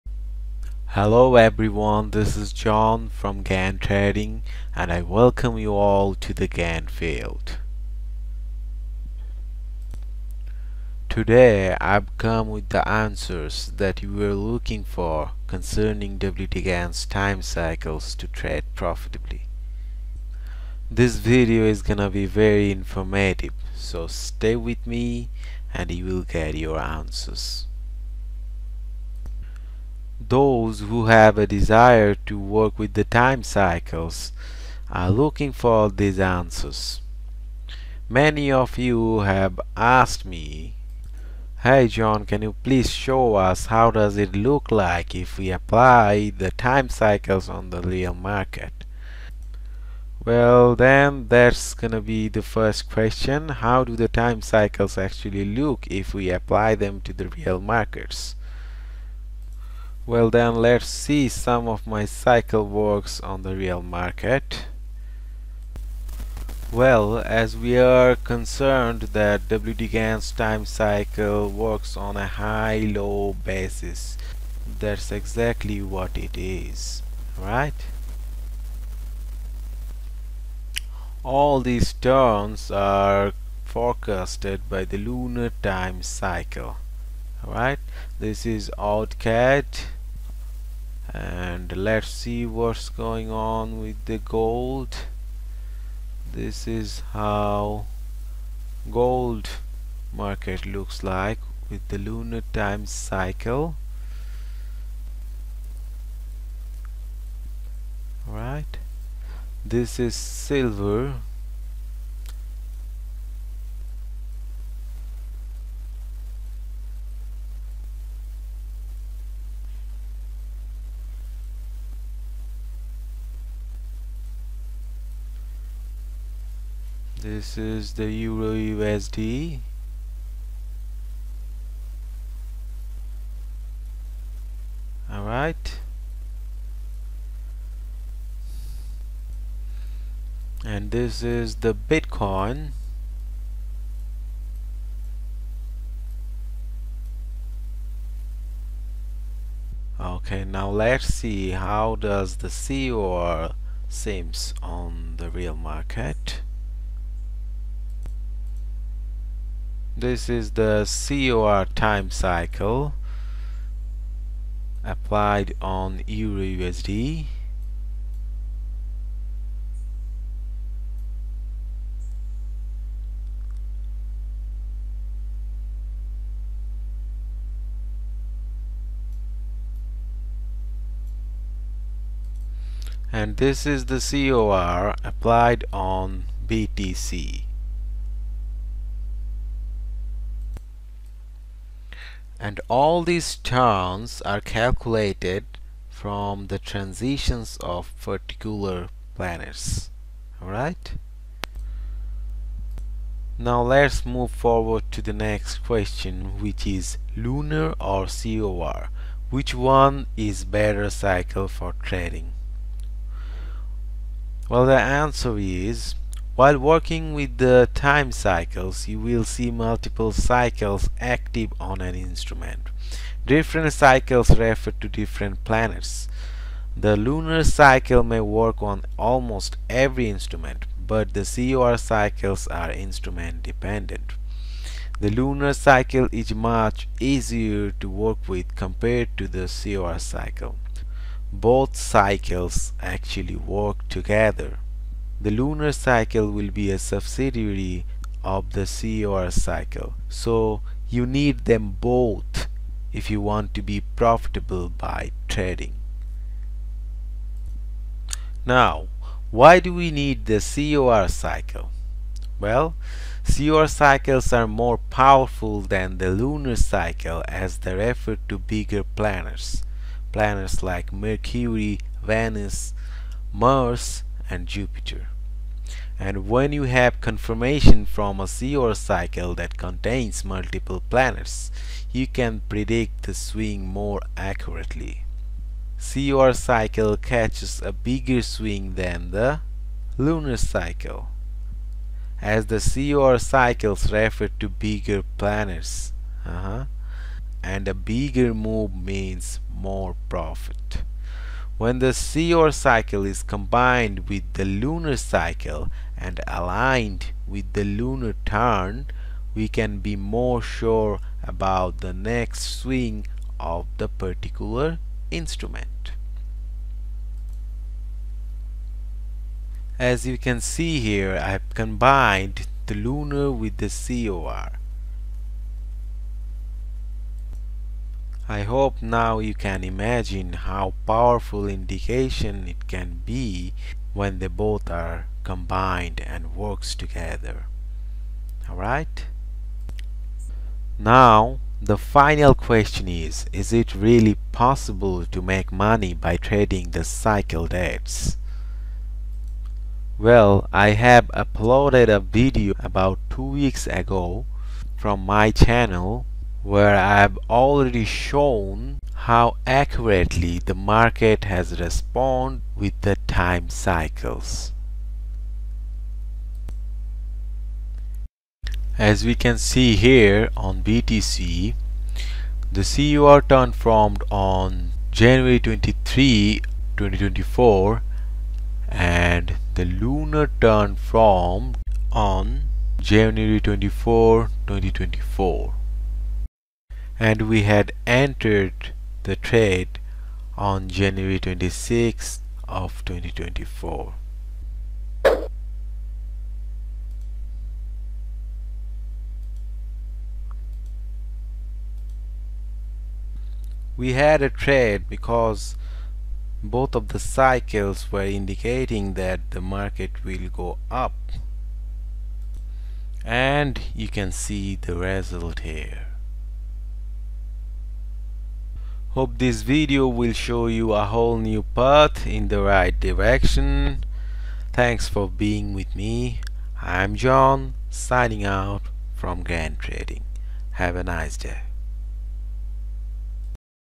WD Gann's time cycles | How does it look in the real market? | EURUSD,BTC,GOLD,SILVER
This video will provide you with the answers that you were looking for concerning WD Gann's time cycle to trade profitably.
Contents of the video:
* Current market observations following the application of the lunar time   cycle and the COR.
* The combined view of the COR and the  lunar time cycle on the real market.
* The significance of the COR time cycle and how the lunar time cycle correlates with it.
* An illustration of how to use the lunar and COR time cycles together to trade like the legend WD Gann.

Timecodes:
0:00 - Intro
0:50 - Real market views with the lunar cycle
2:29 - Real market review with the COR
3:22 - Lunar or COR?
4:17 - Why do we need the COR cycle?
5:17 - Combined view of the Lunar and COR
5:40 - Example of profitability by trading the cycle dates

Time is more important than price - WD Gann.